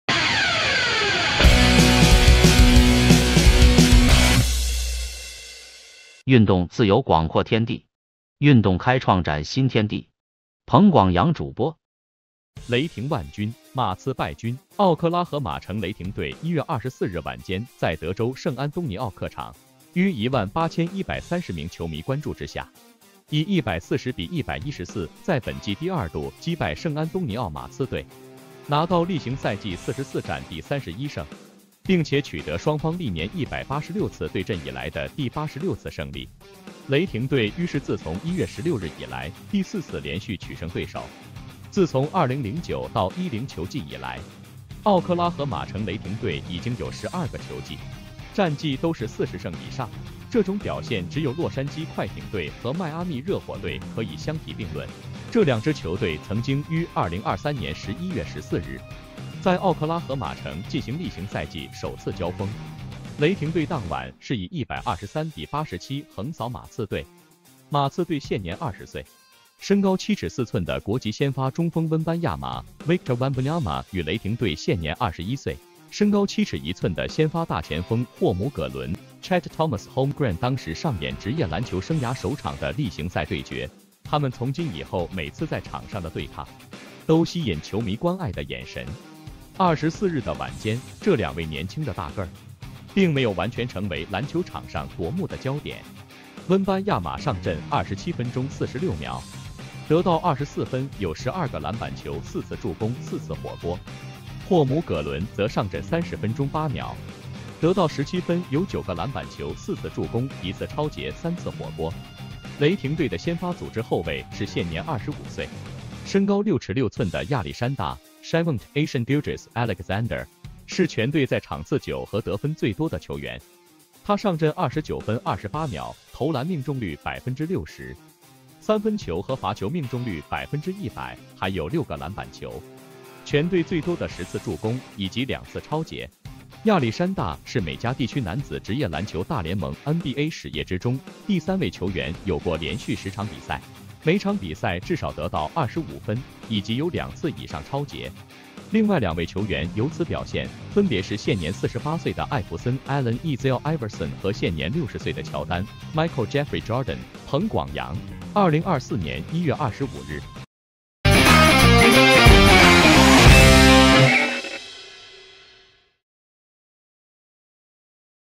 雷霆萬鈞-馬刺敗軍
2024年1月25日

奧克拉荷馬城雷霆隊1月24日晚間在德州聖安東尼奧客場，於1萬8130名球迷關注之下，以140比114在本季第二度擊敗聖安東尼奧馬刺隊，拿到例行賽季44戰第31勝，並且取得雙方歷年186次對陣以來的第86次勝利；雷霆隊於是自從1月16日以來，第4次連續取勝對手。
自從2009-10球季以來，奧克拉荷馬城雷霆隊已經有12個球季，戰績都是40勝以上，這種表現只有洛杉磯快艇隊和邁阿密熱火隊可以相提並論。
這兩支球隊曾經於2023年11月14日，在奧克拉荷馬城進行例行賽季首次交鋒，雷霆隊當晚是以123比87橫掃馬刺隊。
馬刺隊現年20歲，身高7呎4吋的國籍先發中鋒溫班亞瑪(Victor Wembanyama)與雷霆隊現年21歲，身高7呎1吋的先發大前鋒霍姆葛倫(Chet Thomas Holmgren)當時上演職業籃球生涯首場的例行賽對決；他們從今以後每次在場上的對抗，都吸引球迷關愛的眼神。
24日的晚間，這兩位年輕的大個兒，並沒有完全成為籃球場上奪目的焦點；溫班亞瑪上陣27分鐘46秒，得到24分有12個籃板球4次助攻4次火鍋。霍姆葛倫則上陣30分鐘8秒，得到17分有9個籃板球4次助攻1次抄截3次火鍋。
雷霆隊的先發組織後衛是現年25歲，身高6呎6吋的亞歷山大(Shaivonte Aician Gilgeous-Alexander)，是全隊在場次久和得分最多的球員；他上陣29分28秒，投籃命中率60％，三分球和罰球命中率100%，還有6個籃板球，全隊最多的10次助攻，以及2次抄截。
亞歷山大是美加地區男子職業籃球大聯盟(NBA)史頁之中，第3位球員有過連續10場比賽，每場比賽至少得到25分，以及有2次以上抄截。另外兩位球員有此表現，分別是現年48歲的艾佛森(Allen Ezail Iverson)和現年60歲的喬丹(Michael Jeffrey Jordan)。